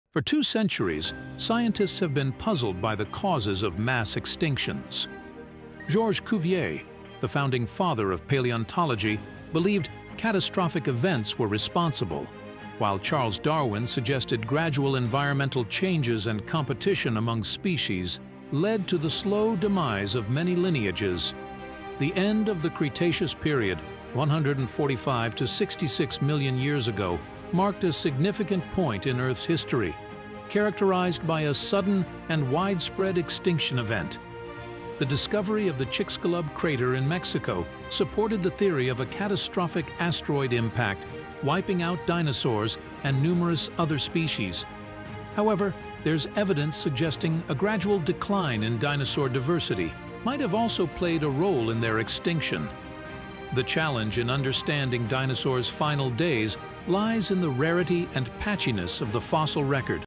Unveiling Africa's Lost Dinosaurs: A Prehistoric Mystery Solved
Dive deep into the discovery of Africa's last dinosaurs and their prehistoric world.